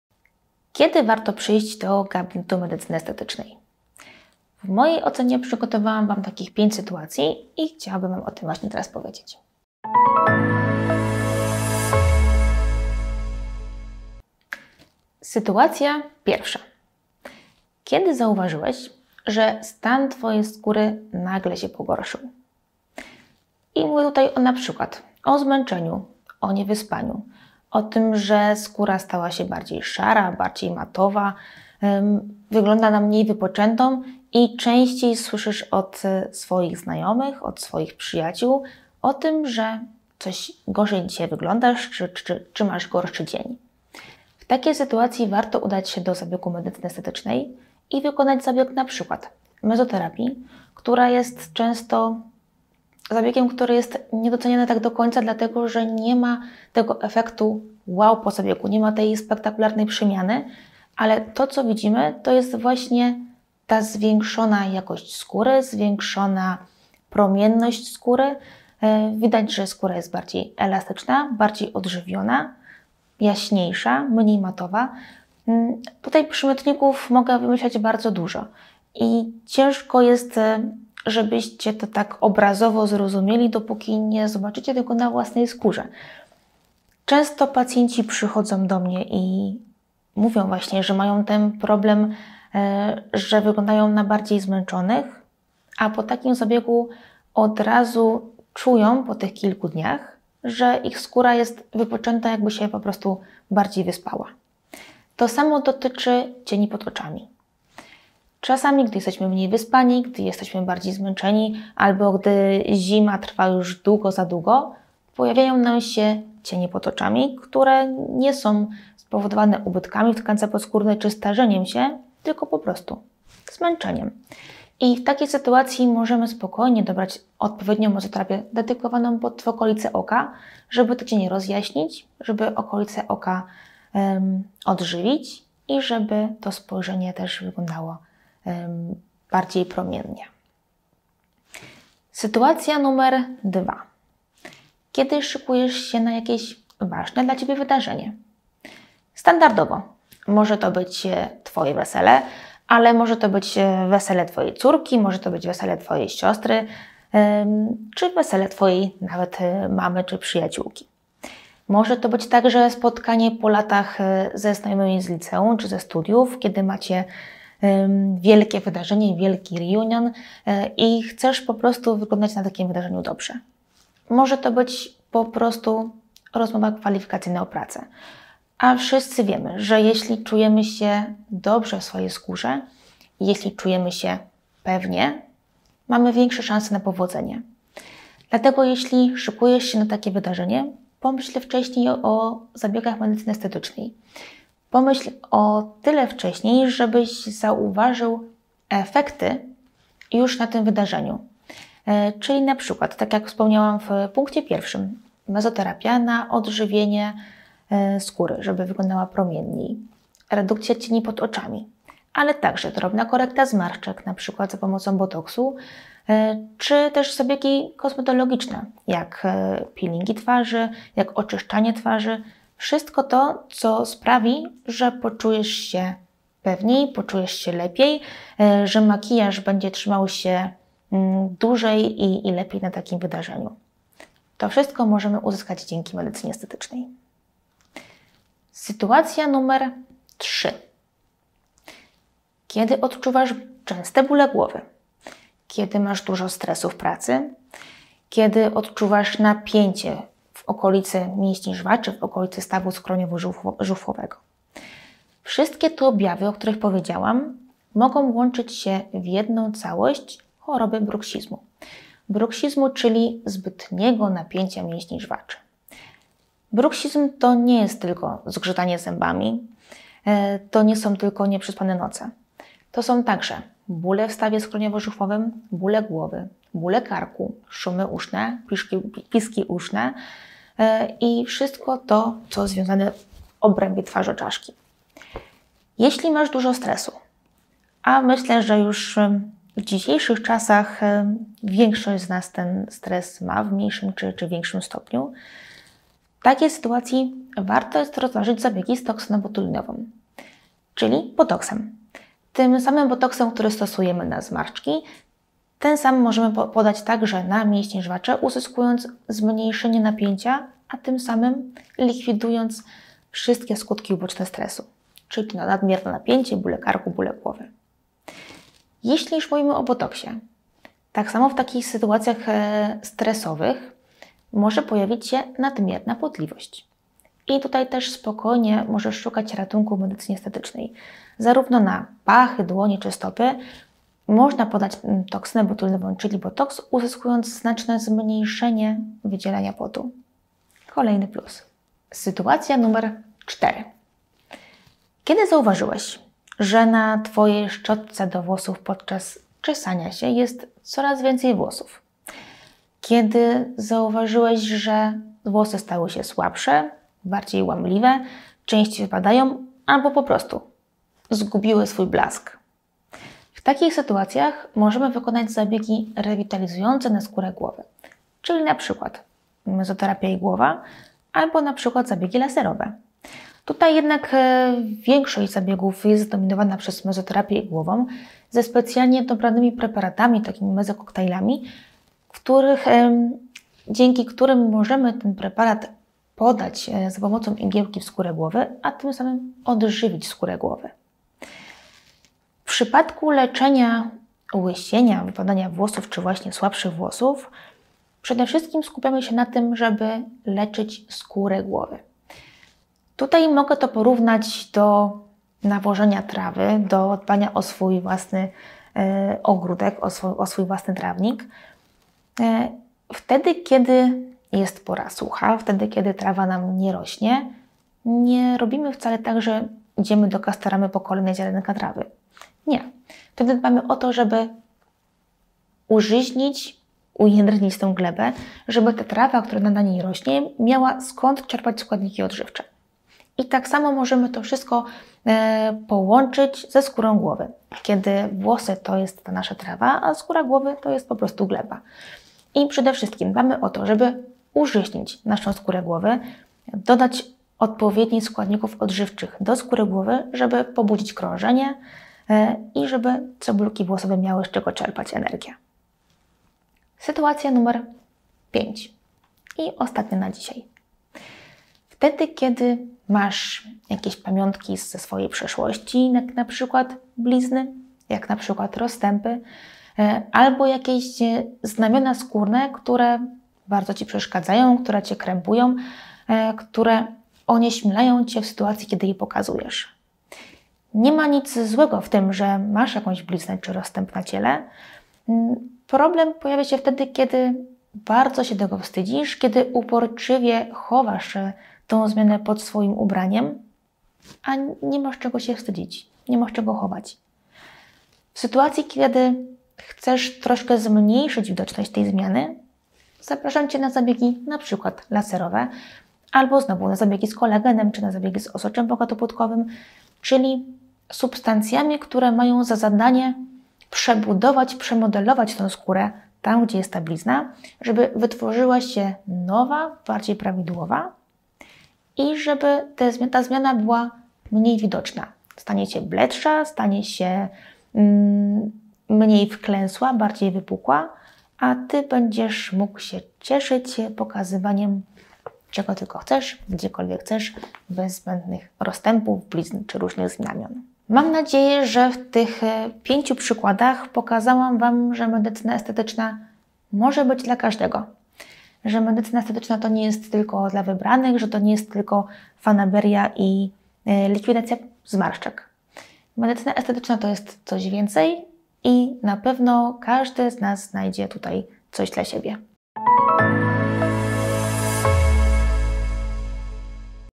Masz Tak? Wtedy PRZYJDŹ DO GABINETU
Jeśli medycyna estetyczna kojarzy Ci się tylko z usuwaniem zmarszczek to ten filmik jest dla Ciebie!

00:00-00:15 Wstęp
00:16-01:44 Kiedy stan Twojej skóry się pogorszył
01:45-02:20 Kiedy masz cienie pod oczami - mezoterapia
02:21-03:57 Kiedy chcesz wyglądać świetnie na ważne wydarzenie
03:58-05:17 Botoks na bóle głowy
05:18-05:39 Kiedy nadmiernie się pocisz – botoks na nadpotliwość
05:40-07:35 Kiedy masz wypadające i słabe włosy
07:36-08:59 Kiedy masz blizny i rozstępy
09:00-09:27 Medycyna estetyczna kiedy przyjść – podsumowanie
Kiedy warto przyjść do gabinetu medycyny estetycznej? W mojej ocenie przygotowałam parę Wam takich 5 sytuacji i chciałabym Wam o tym
właśnie teraz powiedzieć. Sytuacja pierwsza. Kiedy zauważyłeś, że
stan twojej skóry nagle się pogorszył. I mówię tutaj na przykład:o zmęczeniu, o niewyspaniu. O tym, że skóra stała się bardziej szara,
bardziej matowa, wygląda na mniej wypoczętą i częściej
słyszysz od swoich znajomych, od swoich przyjaciół o tym, że coś gorzej dzisiaj wyglądasz, czy masz gorszy dzień.

W takiej sytuacji warto udać się do gabinetu medycyny estetycznej i wykonać zabieg np. mezoterapii, która jest często zabiegiem, który jest niedoceniany tak do końca dlatego, że nie ma tego efektu WOW po zabiegu, nie ma tej spektakularnej przemiany. Ale to co widzimy, to jest właśnie ta zwiększona jakość skóry, zwiększona promienność skóry.

Widać, że skóra jest bardziej elastyczna, bardziej ożywiona, jaśniejsza, mniej matowa. Tutaj przymiotników mogę wymyślać bardzo dużo. I ciężko jest żebyście to tak obrazowo zrozumieli dopóki nie zobaczycie tego na własnej skórze. Często Pacjenci przychodzą do mnie i mówią, że mają ten problem, że wyglądają na bardziej zmęczonych. A po takim zabiegu od razu czują po tych kilku dniach, że ich skóra jest wypoczęta jakby się po prostu bardziej wyspała.

To samo dotyczy cieni pod oczami. Czasami, gdy jesteśmy mniej wyspani, gdy jesteśmy bardziej zmęczeni albo gdy zima trwa już długo za długo, pojawiają nam się cienie pod oczami, które nie są spowodowane ubytkami w tkance podskórnej czy starzeniem się, tylko po prostu - zmęczeniem.

I w takiej sytuacji możemy spokojnie dobrać odpowiednią mezoterapię, dedykowaną pod okolicę oka, żeby to cienie rozjaśnić. Żeby okolicę oka
odżywić i żeby to spojrzenie też wyglądało bardziej promiennie. Sytuacja numer 2. Kiedy szykujsz się na jakieś ważne dla Ciebie wydarzenie. Standardowo. Może to być Twoje wesele, ale może to być wesele Twojej córki. Może to być wesele Twojej siostry czy wesele Twojej mamy czy przyjaciółki. Może to być także spotkanie po latach ze znajomymi z liceum czy ze studiów. Kiedy macie wielkie wydarzenie i wielki Reunion i chcesz po prostu wyglądać na takim wydarzeniu dobrze.

Może to być po prostu rozmowa kwalifikacyjna o pracę. A wszyscy wiemy, że jeśli czujemy się dobrze w swojej skórze, jeśli czujemy się pewnie, mamy większe szanse na powodzenie. Dlatego jeśli szykujesz się takie wydarzenie, pomyśl wcześniej o zabiegach medycyny estetycznej. Pomyśl o tyle wcześniej, żebyś zauważył efekty już na tym wydarzeniu.

Czyli na przykład, tak jak wspomniałam w punkcie 1. Mezoterapia na odżywienie skóry - żeby wyglądała promienniej. Redukcja cieni pod oczami.
Ale także - drobna korekta zmarszczek, na przykład za pomocą botoksu.
Czy też zabiegi kosmetologiczne. Jak peelingi twarzy, jak oczyszczanie twarzy. Wszystko to, co sprawi, że poczujesz się pewniej. Poczujesz się lepiej. Że makijaż będzie trzymał się dłużej i lepiej na takim wydarzeniu. To wszystko możemy uzyskać dzięki medycynie estetycznej.

Sytuacja numer 3. Kiedy odczuwasz częste bóle głowy. Kiedy masz dużo stresu w pracy. Kiedy odczuwasz napięcie w okolicy mięśni żwaczy,
w okolicy stawu skroniowo-żuchwowego. Wszystkie te objawy, o których powiedziałam mogą łączyć się w jedną całość choroby bruksizmu. Bruksizmu, czyli zbytniego napięcia mięśni żwaczy. Bruksizm to nie jest tylko zgrzytanie zębami. To nie są tylko nieprzespane noce.
To są także: bóle w stawie skroniowo-żuchwowym. Bóle głowy, bóle karku, szumy uszne, piski uszne i wszystko to, co związane w obrębie  twarzoczaszki. Jeśli masz dużo stresu - a myślę, że już w dzisiejszych czasach większość z nas ten stres ma, w mniejszym czy większym stopniu, w takiej sytuacji warto jest rozważyć zabiegi z toksyną botulinową czyli - botoksem. Tym samym botoksem, który stosujemy na zmarszczki.